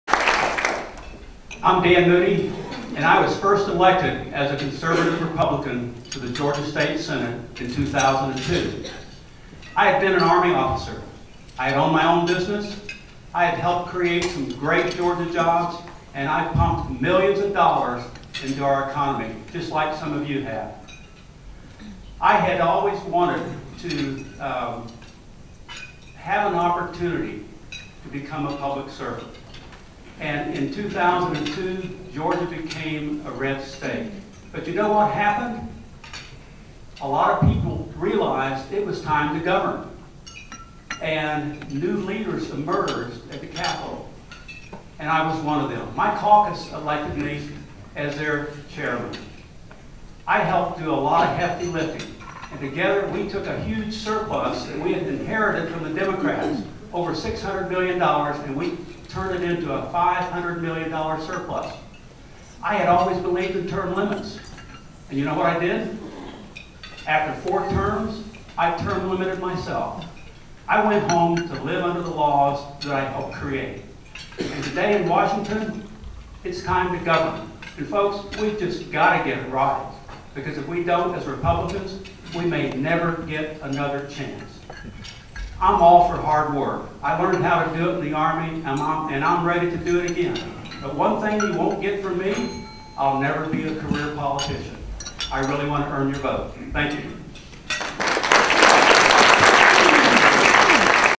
Dan Moody – Republican candidate for Congress for Georgia’s 6th District 03/29/17
Dan Moody – Republican candidate for Congress for Georgia’s 6th District

I'm Dan Moody and I was first elected as a conservative Republican to the Georgia State Senate in 2002.

I had been an Army officer. I had owned my own business. I had helped create some great Georgia jobs and I pumped millions of dollars into our economy — just like some of you have.

I had always wanted to, um, have an opportunity to become a public servant and in 2002, Georgia became a red state, but you know what happened? A lot of people realized it was time to govern and new leaders emerged at the Capitol and I was one of them.

My caucus elected me as their chairman. I helped do a lot of hefty lifting and together we took a huge surplus we had inherited from the Democrats over six hundred million dollars and we turned it into a five hundred million dollar surplus.

I had always believed in term limits, and you know what I did? After four terms, I term-limited myself. I went home to live under the laws that I helped create.

And today in Washington, it's time to govern, and folks, we've just got to get it right because if we don't as Republicans, we may never get another chance.

I'm all for hard work. I learned how to do it in the Army and I'm ready to do it again, but one thing you won't get from me — I'll never be a career politician.

I really want to earn your vote. Thank you.

Special election Tuesday, April 18, 2017 at 7am-7pm
Early voting till Friday, April 14, 2017 at 8:30am-6pm

Candidate Forum hosted by BrandBank and moderated by Wes Moss at the Cobb Energy Performing Arts Centre in Atlanta, Georgia on Wednesday, March 29, 2017 at 12 noon.